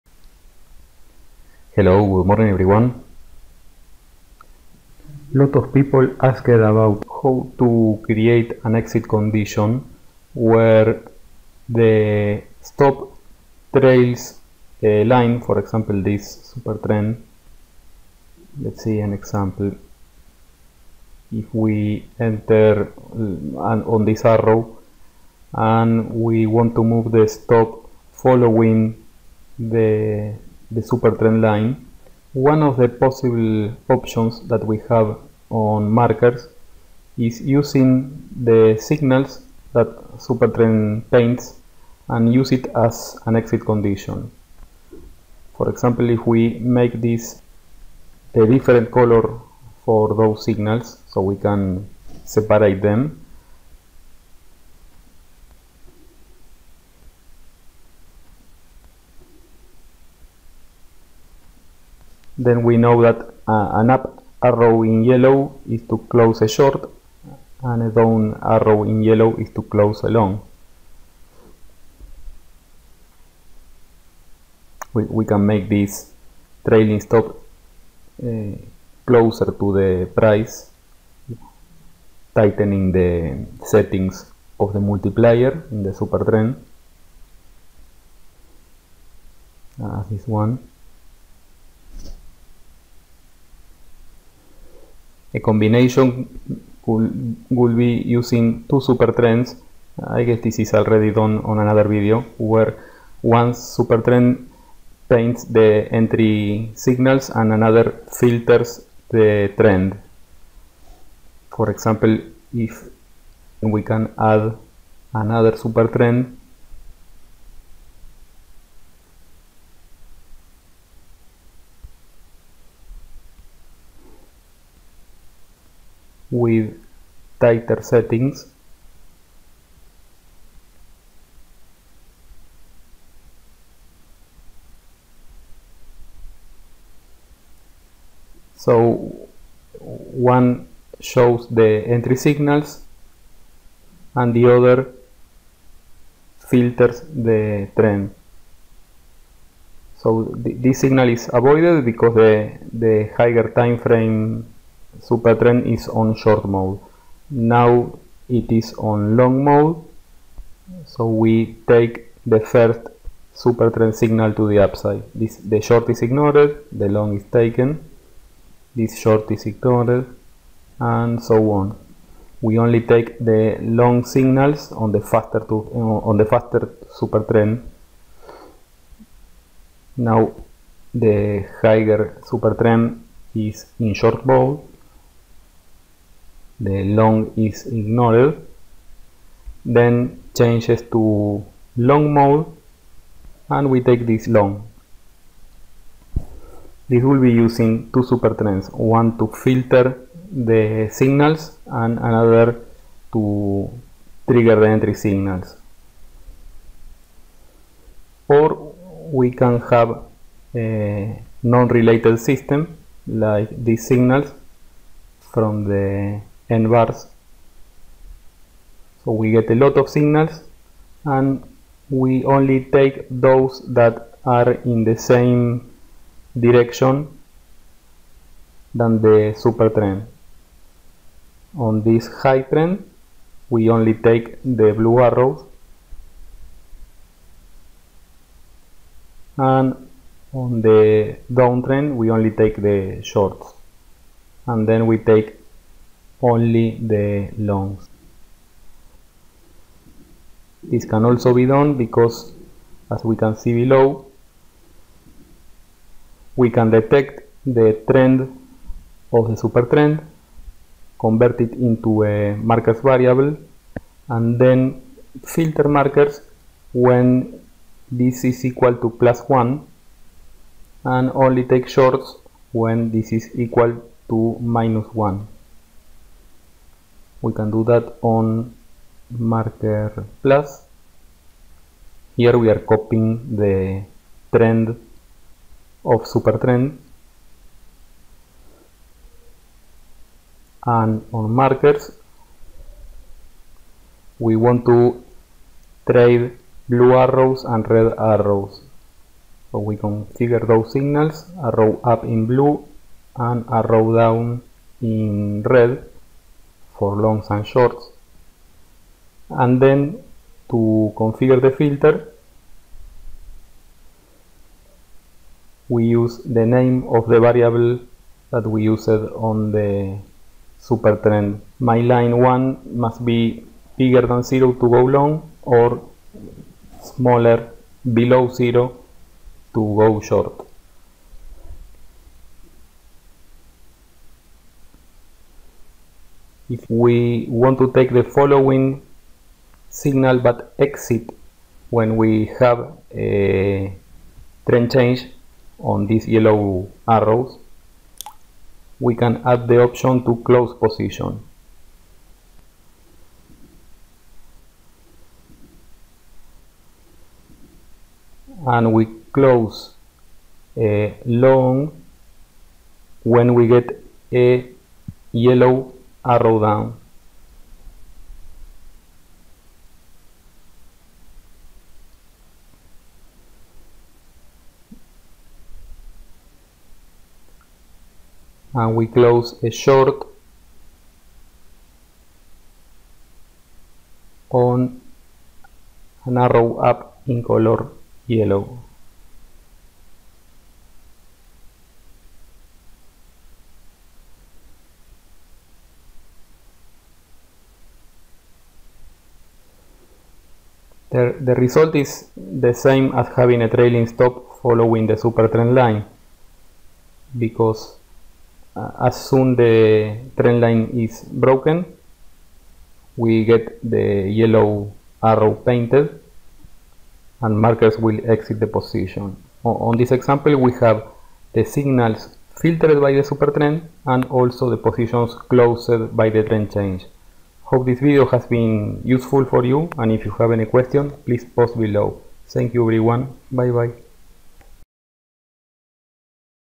How to define Exit Signals in our Trading Setup
On this video I explain how to filter signals and how to configure Exit Signals in our trade setup using Markers Plus Tools for NinjaTrader